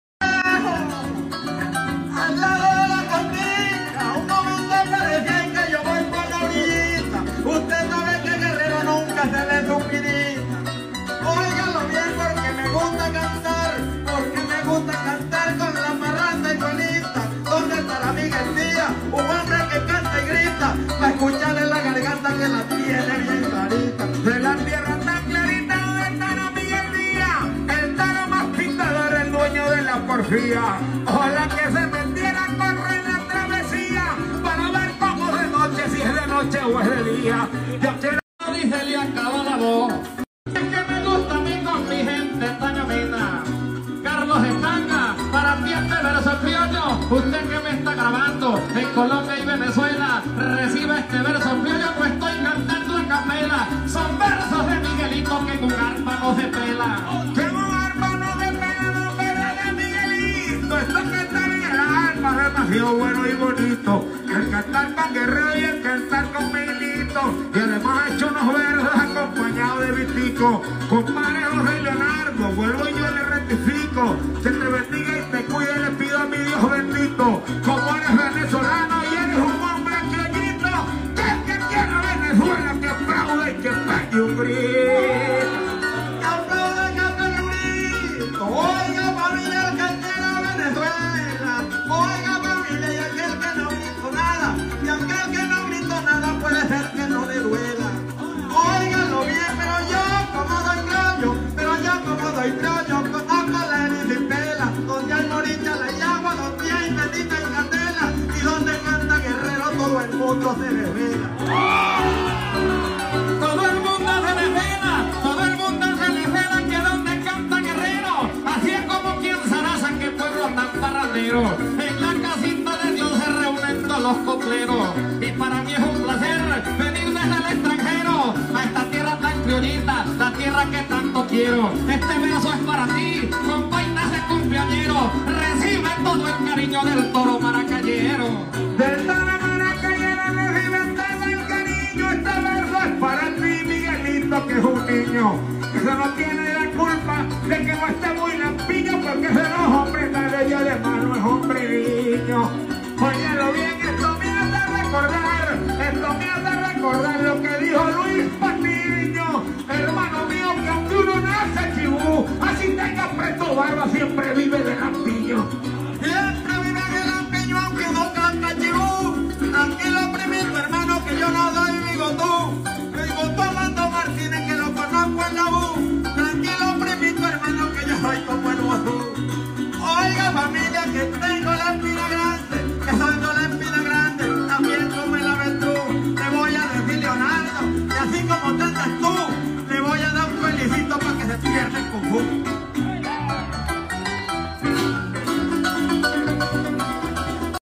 Contrapunteo Jorge Guerrero, Miguelito Díaz, Vitico Castillo en Zaraza
Contrapunteo Jorge Guerrero, Miguelito Díaz, Vitico Castillo en Zaraza CELEBRANDO el Cumpleaños de José Leonardo Lagreca